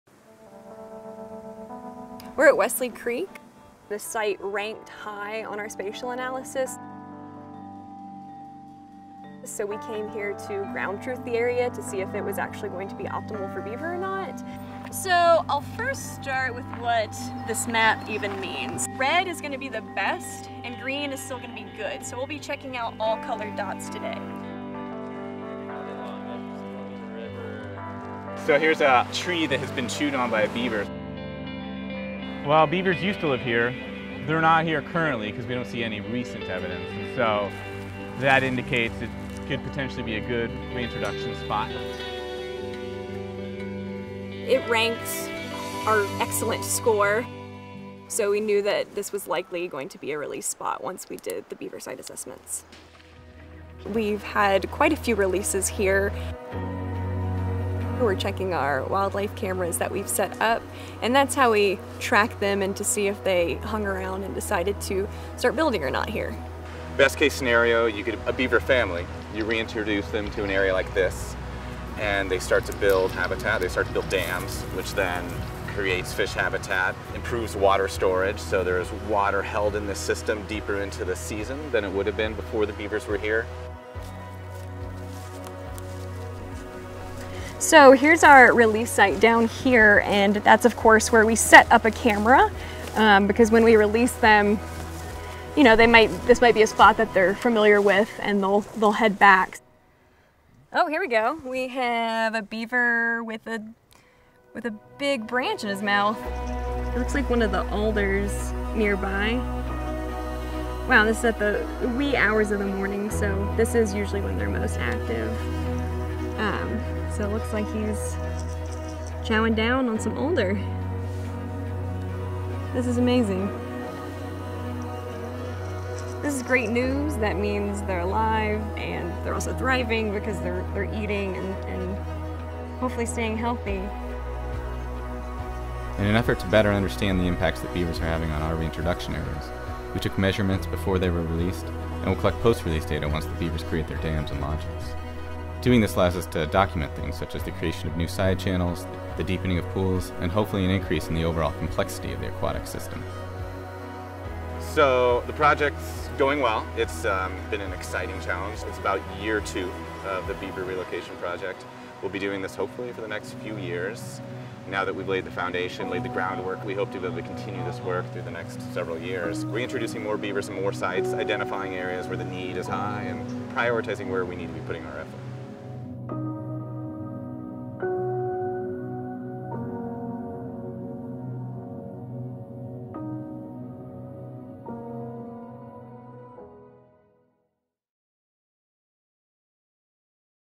Beaver Reintroduction in the Washington Cascades
For the past 3 years, Cascade Forest Conservancy has been engaged in a large-scale beaver reintroduction project in Washington's South Cascades. In 2019 we reintroduced 21 beavers and there are many more to come! Take a look at the link below to learn more about our aquatic restoration programs:

You can support our beaver reintroduction and other restoration and advocacy projects by making a donation